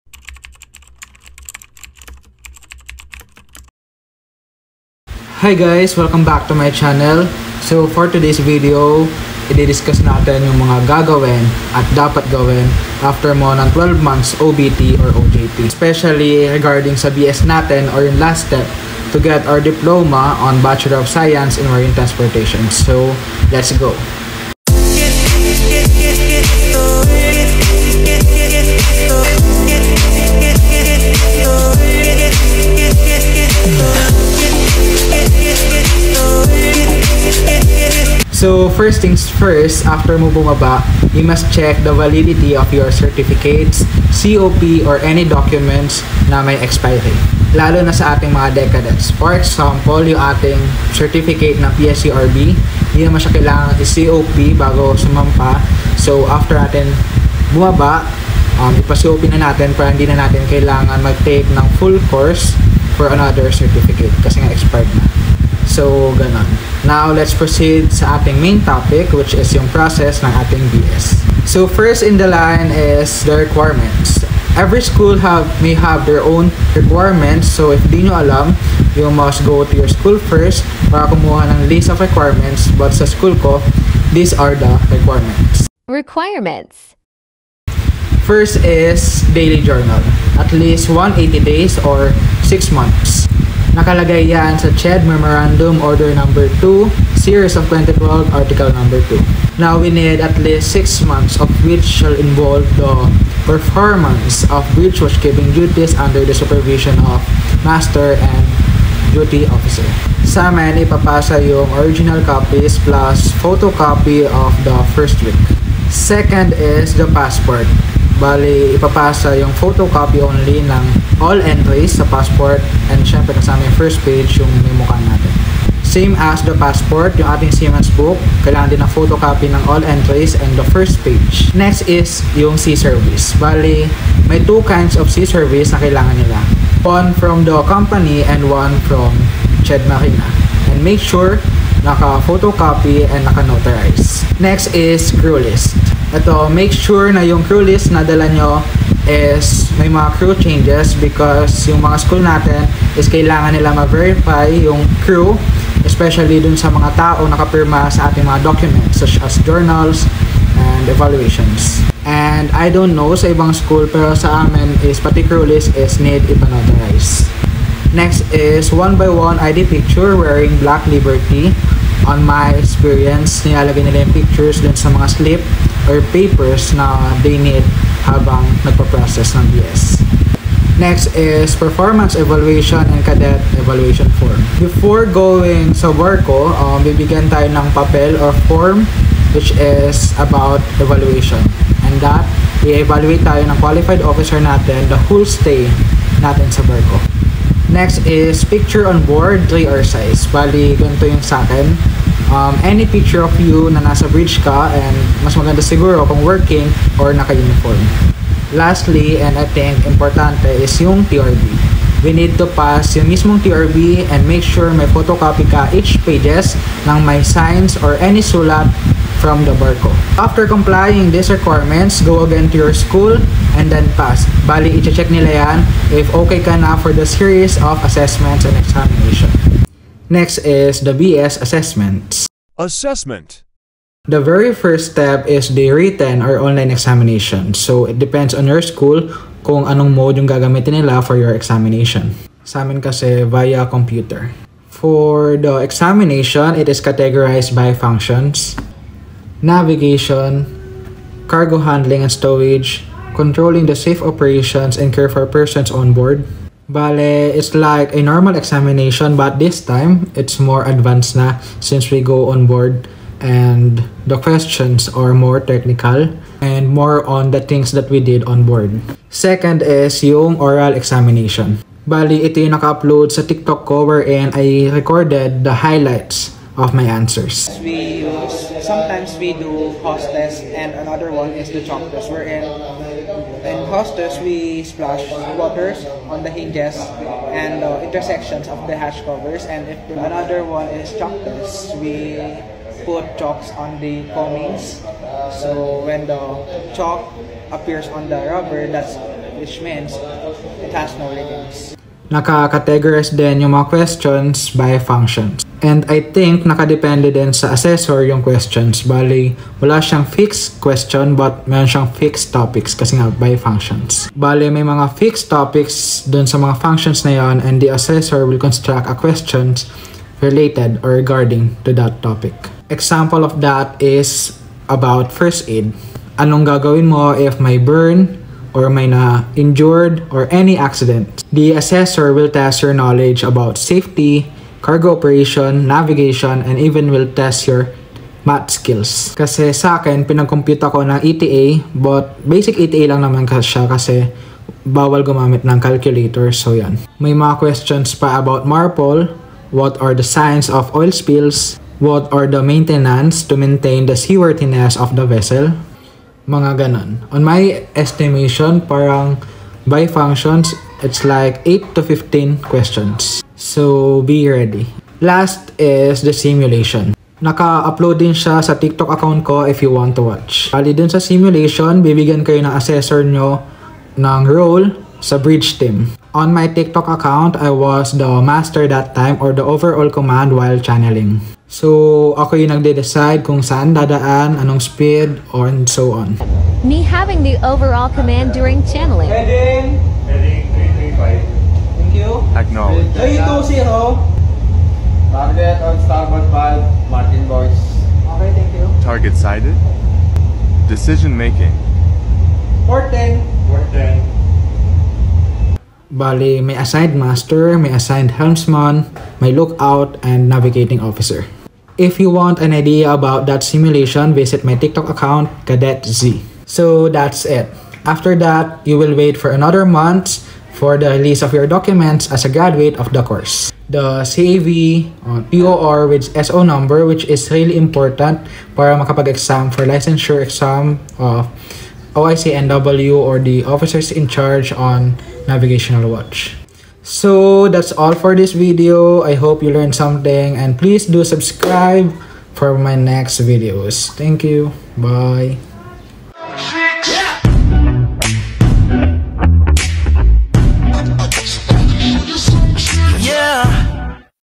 TUTORIAL | Process for BS degree for Marine Transportation ( Requirements to prepare )
Hello mga kabaro, I’m here again sharing my experience on my recent errand. Recently undergo BS assessment to finish my Bachelor degree for Marine Transportation

I hope you learn something! Safe seas and Fair winds!